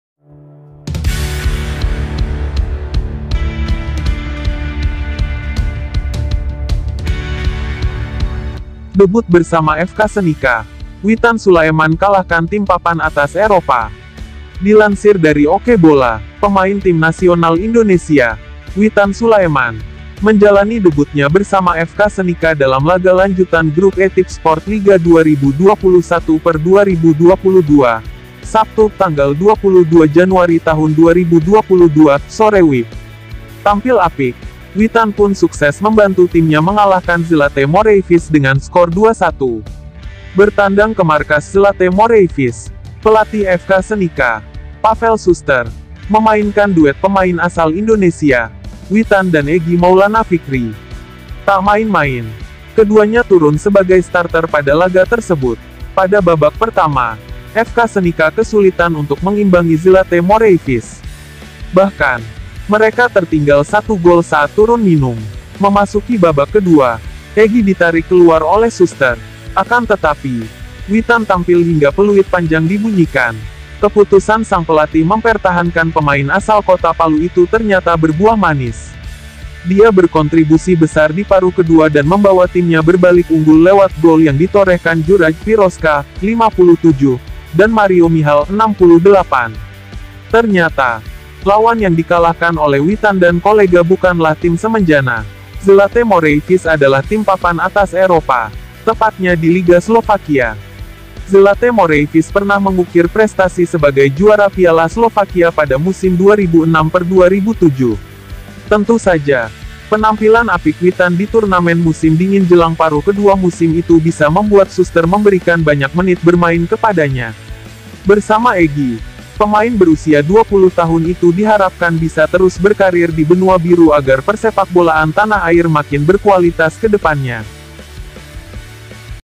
DEBUT PEMAIN TIMNAS INDONESIA WITAN SULAIMAN SUKSES BAWA TIM FK SENICA MENANG DI EROPA
Pemain Tim Nasional Indonesia, Witan Sulaeman, menjalani debutnya bersama FK Senica dalam laga lanjutan Grup E Tipsport Liga 2021/2022, Sabtu (22/1/2022), sore WIB. Tampil apik, Witan pun sukses membantu timnya mengalahkan Zlate Moravce dengan skor 2 1.